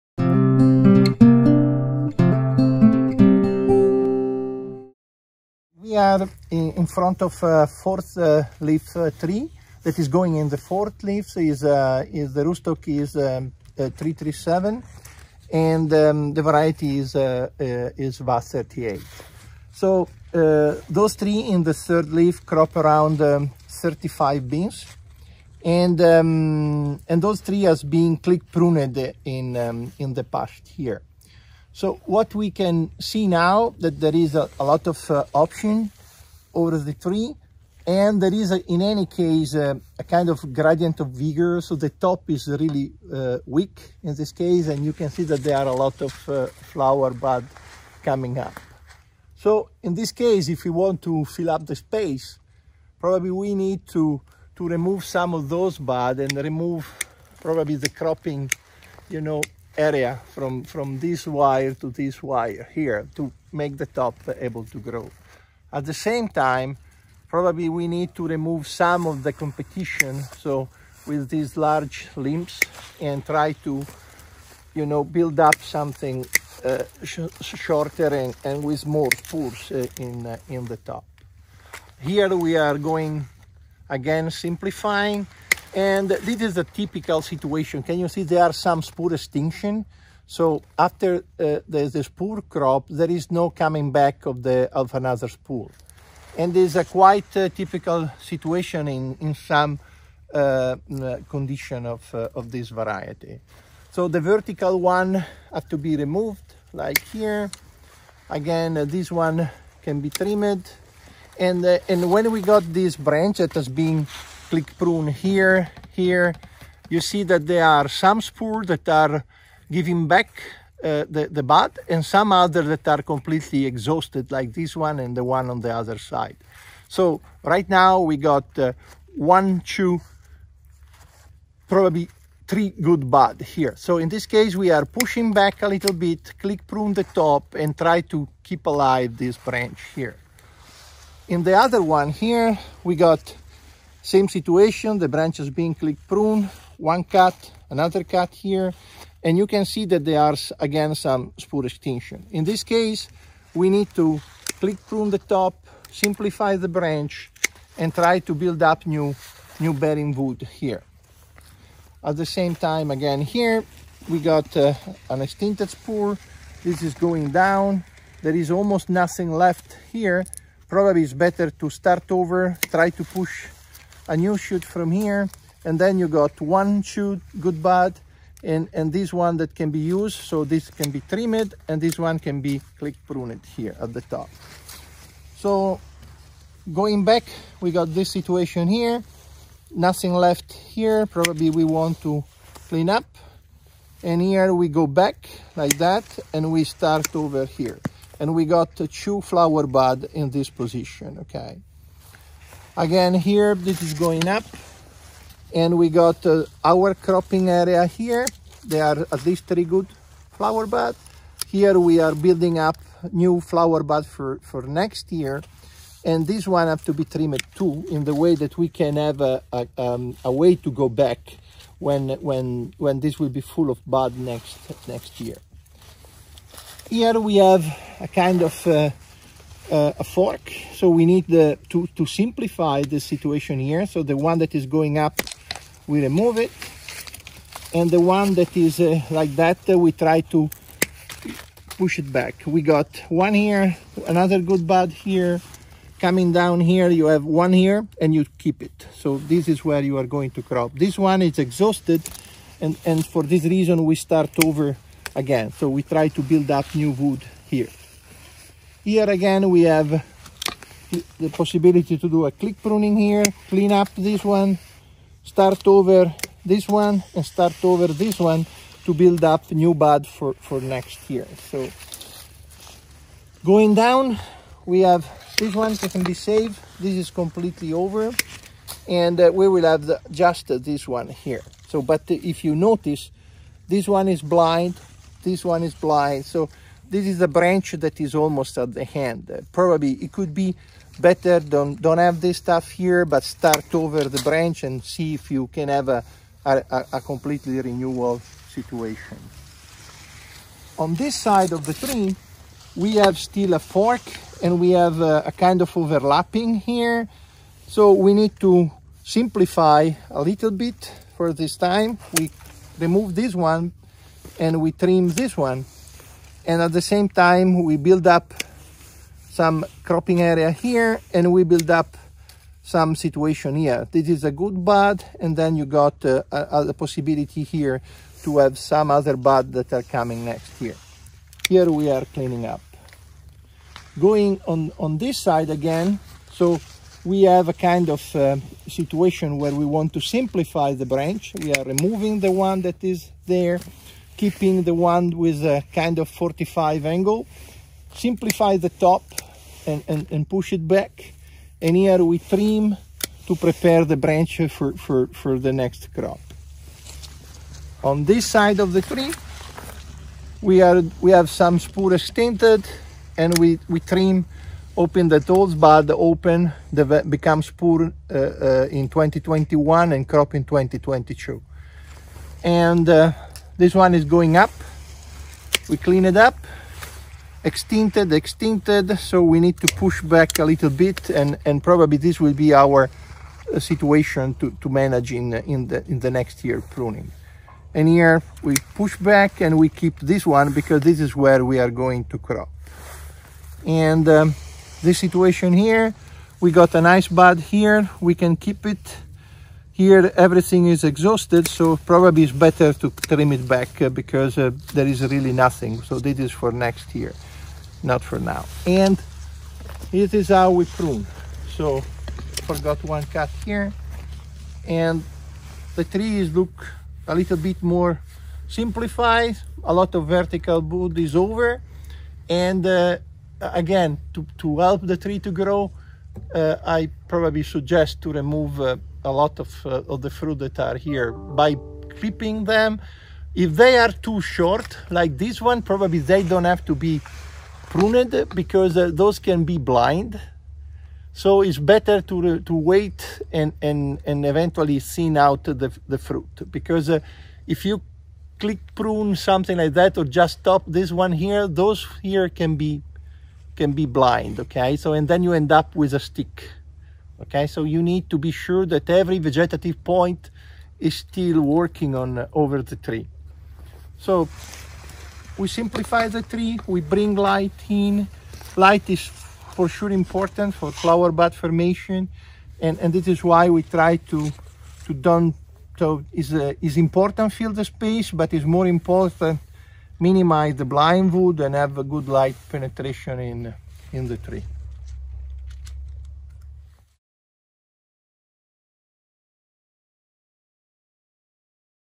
Winter prune fourth leaf WA 38 (Cosmic Crisp)
Washington State University's Stefano Musacchi demonstrates how to winter prune fourth leaf WA 38 (Cosmic Crisp) trees planted on M9-337 in a modern commercial V-trellis system in the Yakima Valley, Washington.